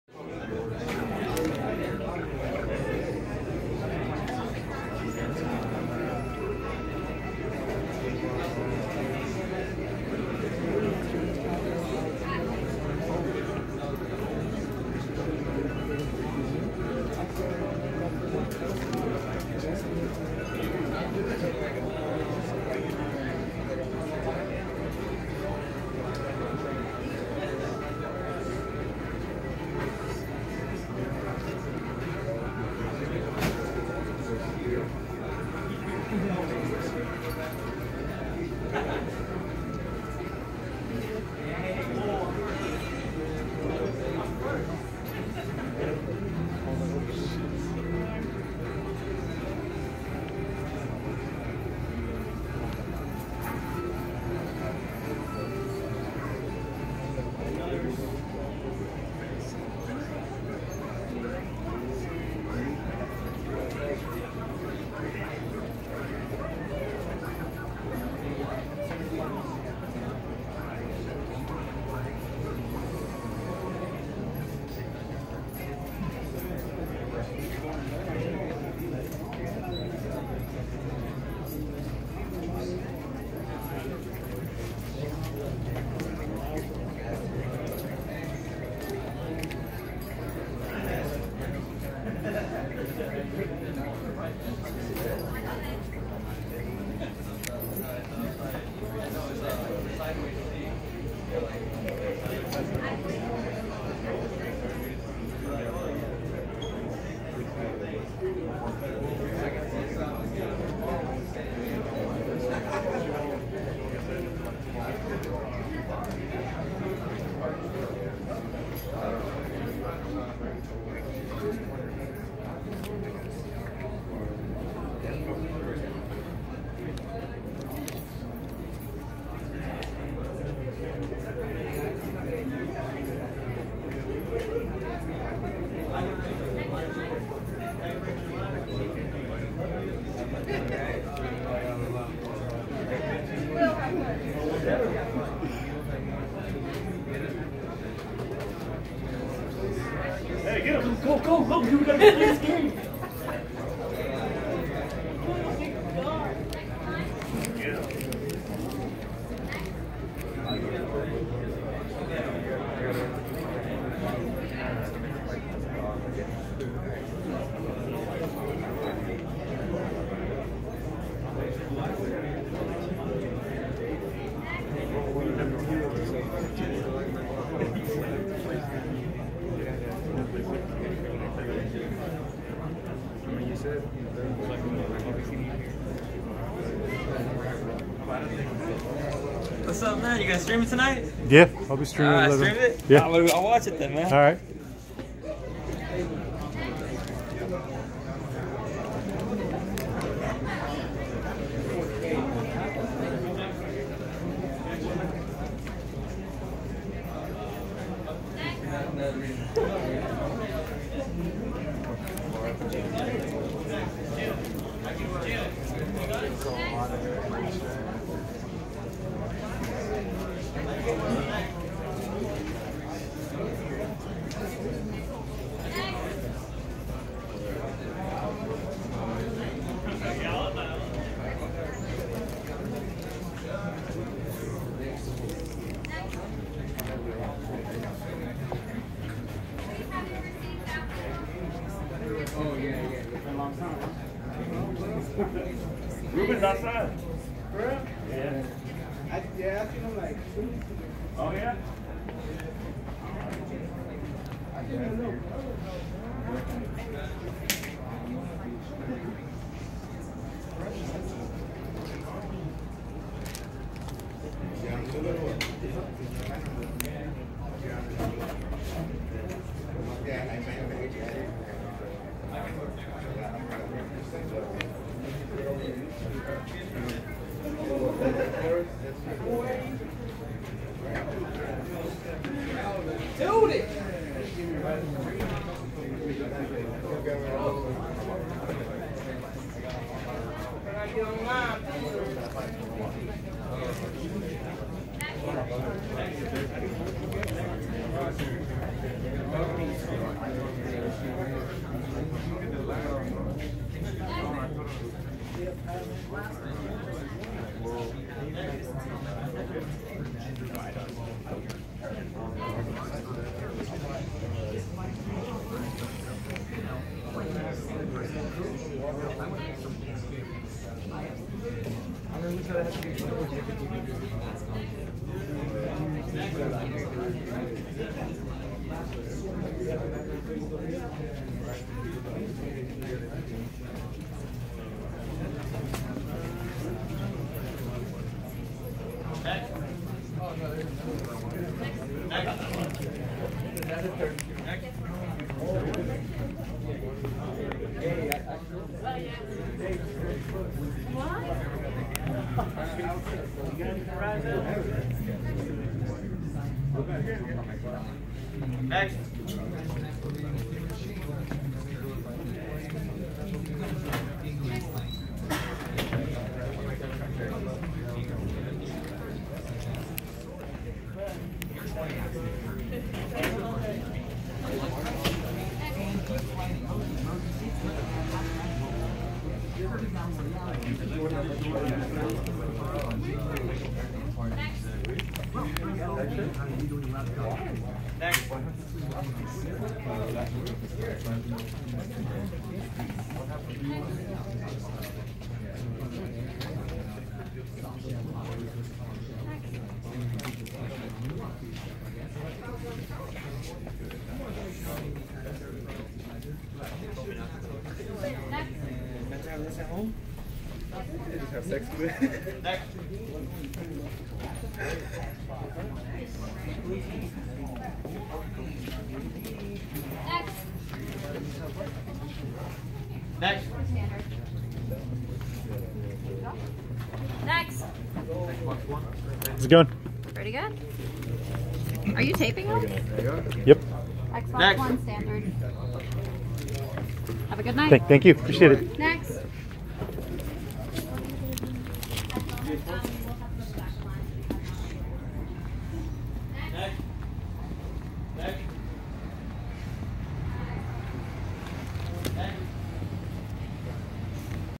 CALL OF DUTY: WORLD WAR II GAMESTOP NIGHT RELEASE 11-2-17
PICKING UP CALL OF DUTY: WWII